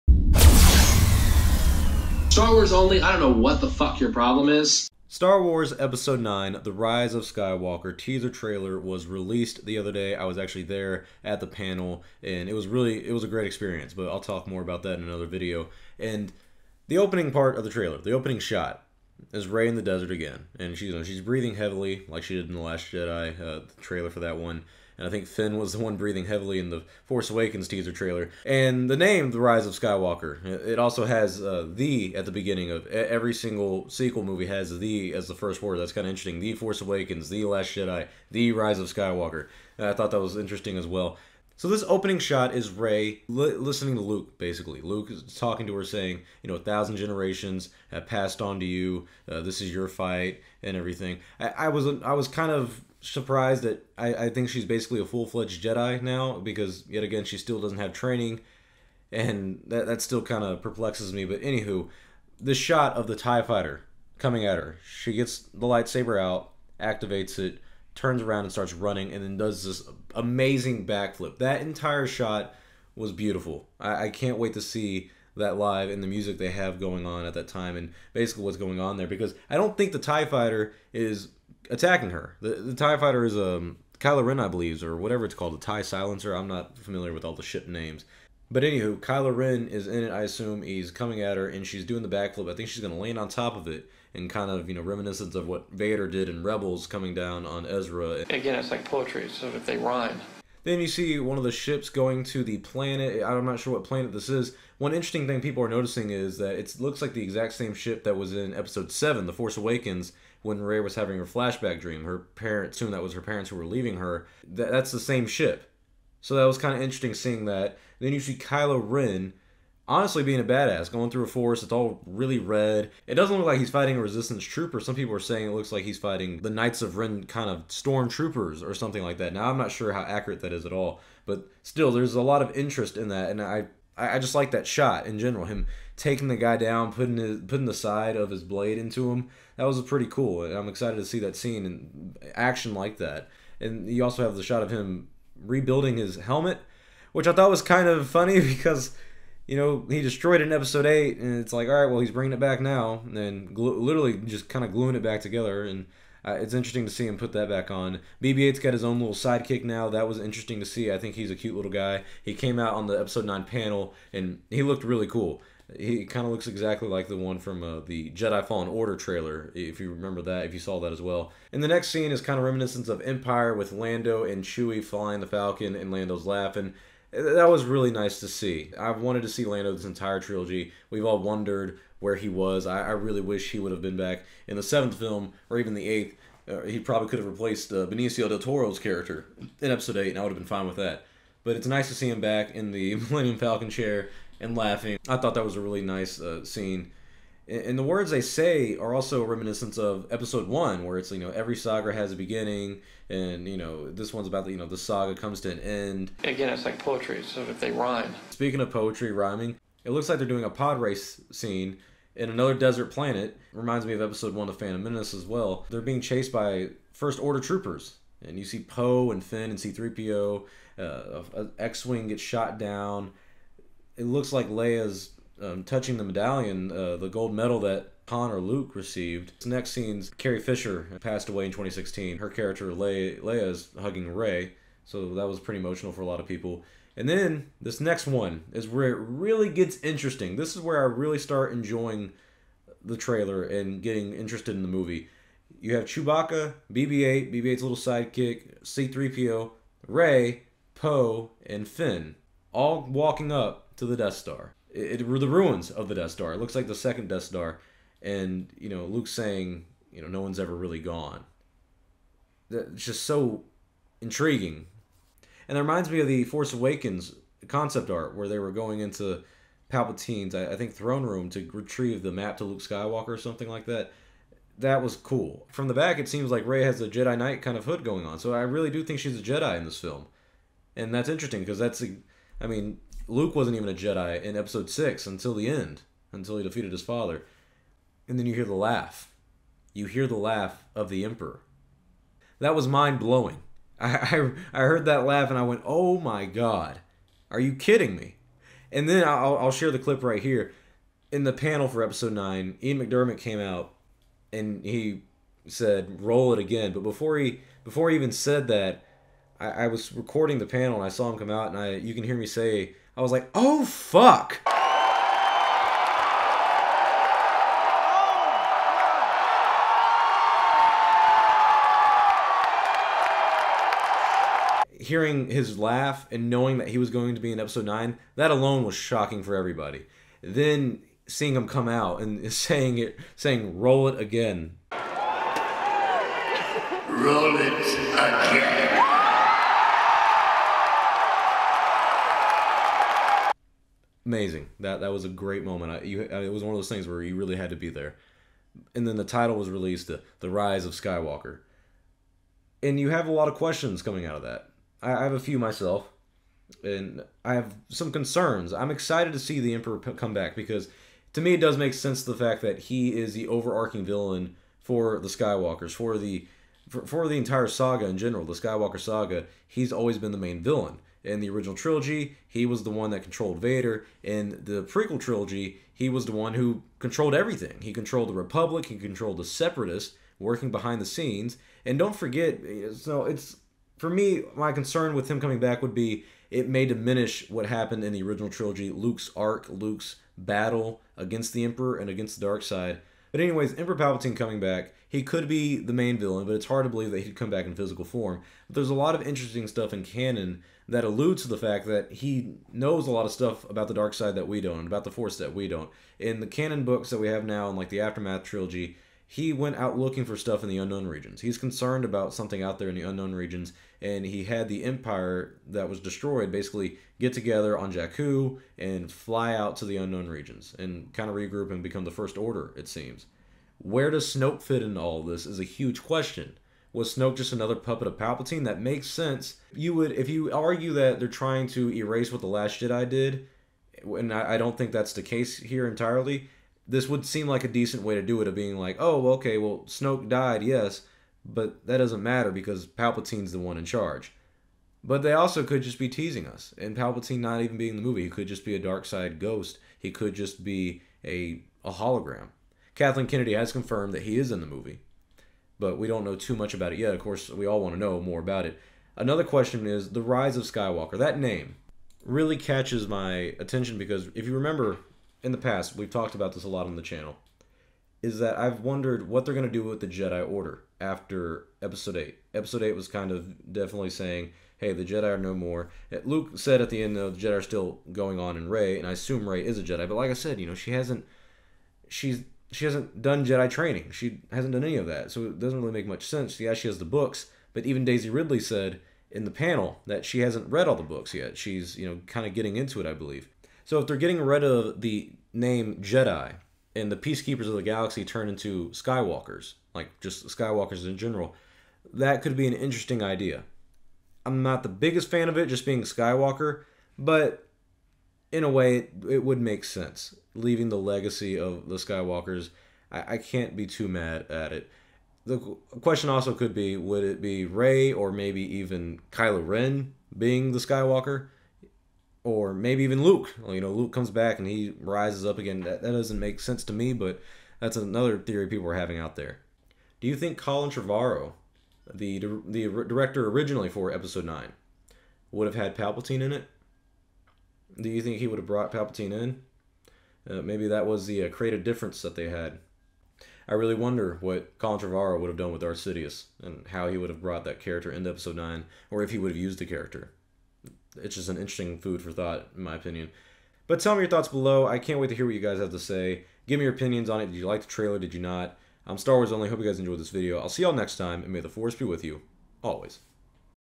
The Rise of Skywalker trailer thoughts
Star Wars Episode 9 the rise of Skywalker released its first long-awaited trailer, in this video I give my thoughts on the trailer and break it down.